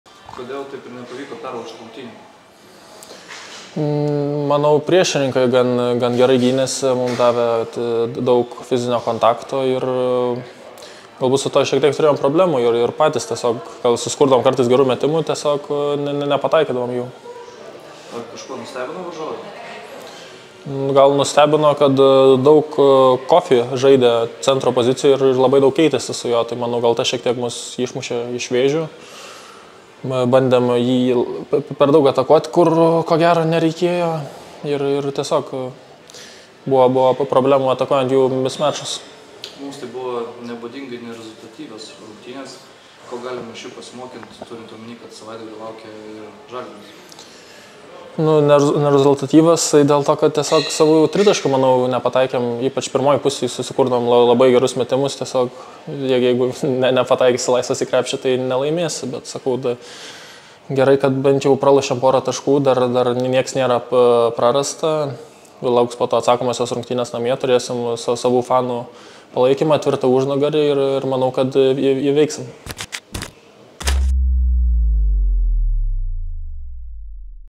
G. Masiulis: „Nepataikysi laisvas į krepšį – nelaimėsi“
Vilniaus „Ryto“ puolėjo komentaras po Patruose pralaimėtų rungtynių prieš „Promitheas“.